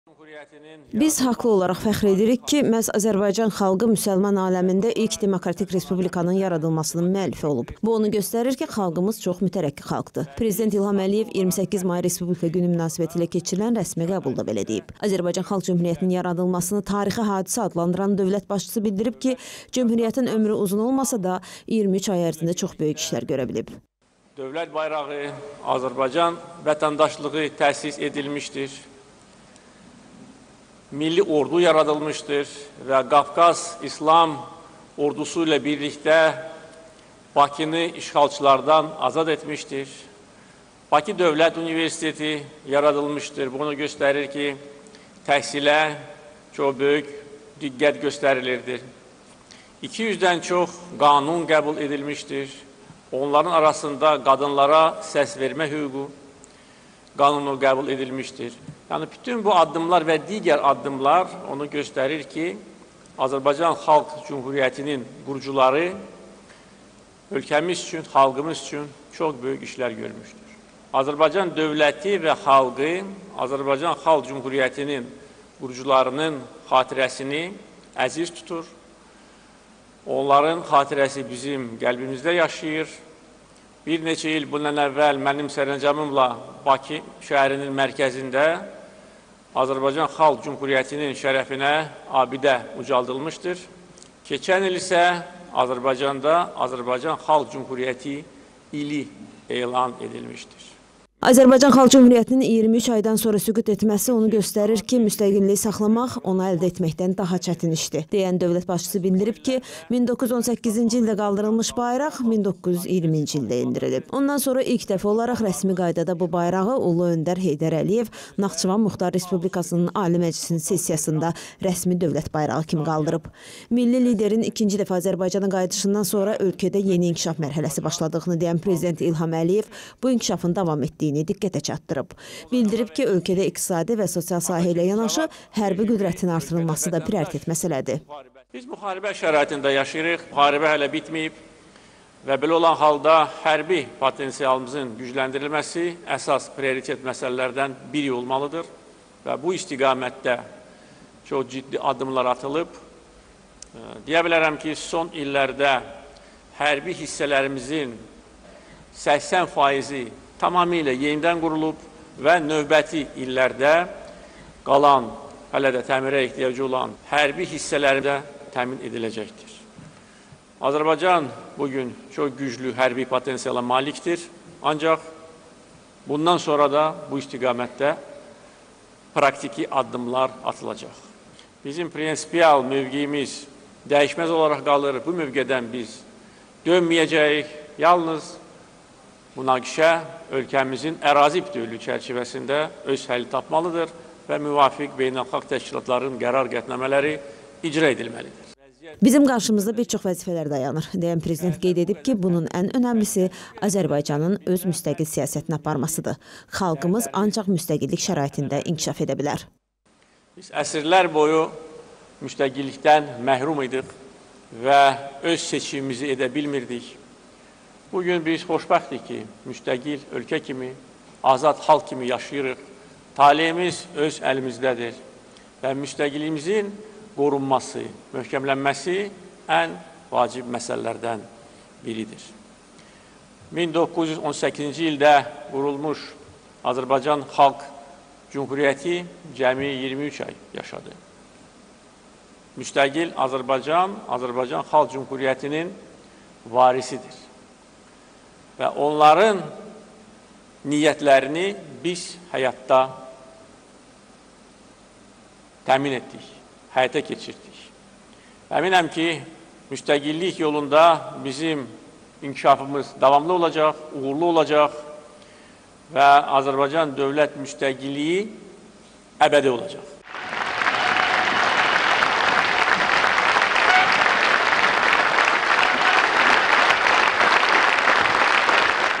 PREZİDENT İLHAM ƏLİYEV: “AZƏRBAYCANIN DÖVLƏT MÜSTƏQİLLİYİ ƏBƏDİ OLACAQ”
Biz haqlı olaraq fəxr edirik ki, məhz Azərbaycan xalqı müsəlman aləmində ilk demokratik respublikanın yaradılmasının müəllifi olub. Bu, onu göstərir ki, xalqımız çox mütərəqqi xalqdır. Prezident İlham Əliyev 28 may Respublika Günü münasibətilə keçirilən rəsmi qəbulda belə deyib. Azərbaycan Xalq Cümhuriyyətinin yaradılmasını tarixi hadisə adlandıran dövlət başçısı bildirib ki, Cümhuriyyətin ömrü uzun olmasa da, 23 ay ərzində çox böyük işlər görə bilib.
Azərbaycan Xalq Cümhuriyyətinin iyirmi üç aydan sonra süqut etməsi  onu göstərir ki, müstəqilliyi saxlamaq onu əldə etməkdən daha çətin işdir – deyən dövlət başçısı bildirib ki, 1918-ci ildə qaldırılmış bayraq 1920-ci ildə endirilib.  Ondan sonra ilk dəfə olaraq rəsmi qaydada bu bayrağı ulu öndər Heydər Əliyev Naxçıvan Muxtar Respublikasının Ali Məclisinin sessiyasında rəsmi dövlət bayrağı kimi qaldırıb. Milli liderin ikinci dəfə Azərbaycana qayıdışından sonra ölkədə yeni inkişaf mərhələsi başladığını deyən prezident İlham Əliyev bu inkişafın davam etdiyini diqqətə çatdırıb. Bildirib ki, ölkədə iqtisadi və sosial sahə ilə yanaşı hərbi qüdrətinin artırılması prioritet məsələdir.
Bizim qarşımızda bir çox vəzifələr dayanır – deyən prezident qeyd edib ki, bunun ən önəmlisi Azərbaycanın öz müstəqil siyasətini aparmasıdır. Xalqımız ancaq müstəqillik şəraitində inkişaf edə bilər.